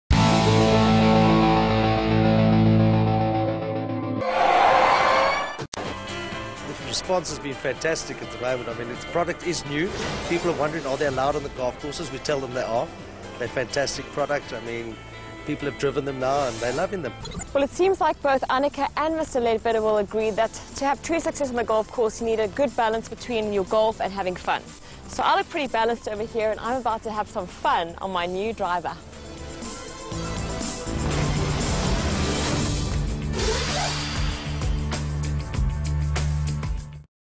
SA Scooter Shop Golf Cruiser!
The latest addition here at SA Scooter Shop - Save costs on hiring a cart every time you visit the green! Taking South African golf courses by storm, the golf cruiser is right on par!